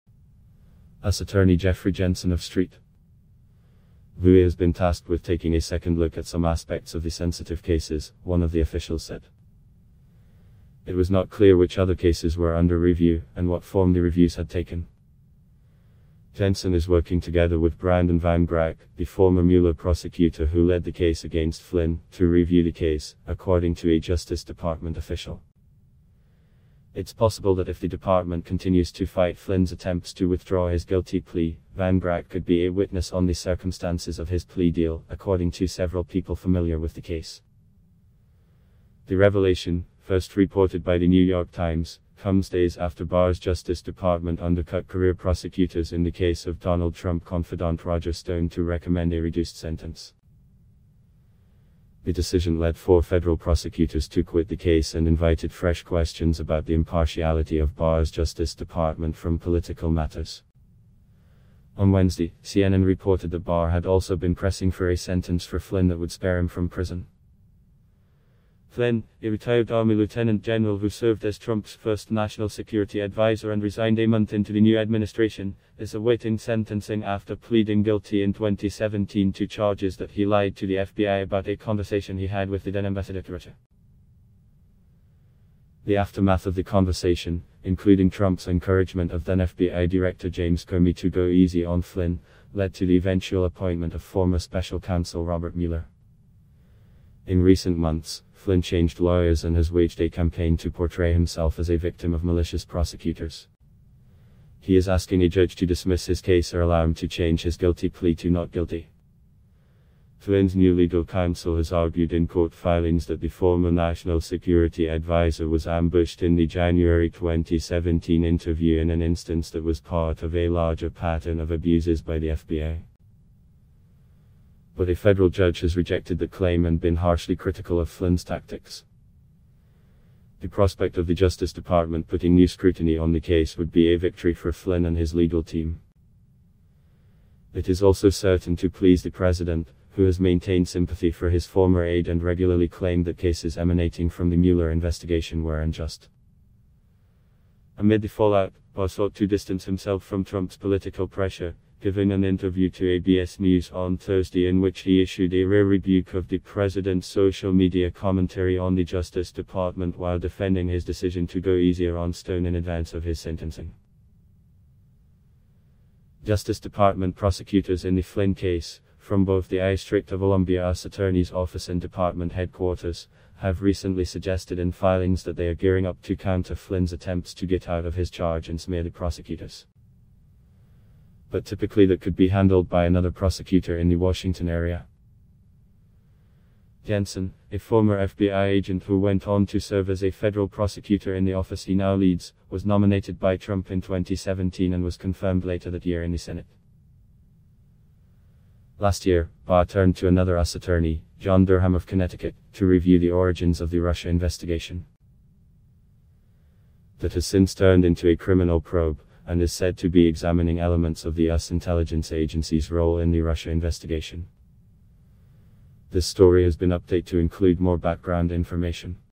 Barr ordered reexamination of Flynn's case, US officials say
US Attorney Jeffrey Jensen of St. Louis has been tasked with taking a second look at some aspects of the sensitive cases, one of the officials said.